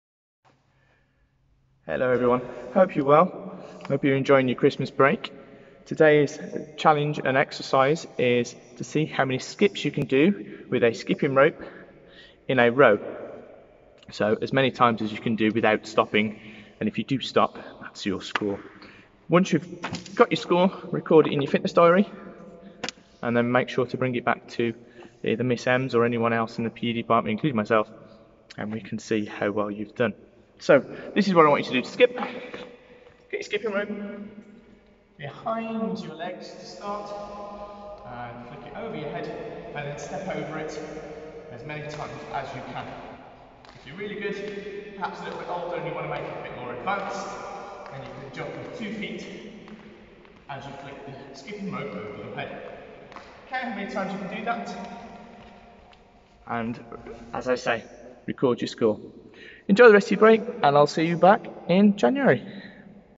Day 8: Skipping Challenge
How many times can you skip with a rope in a row?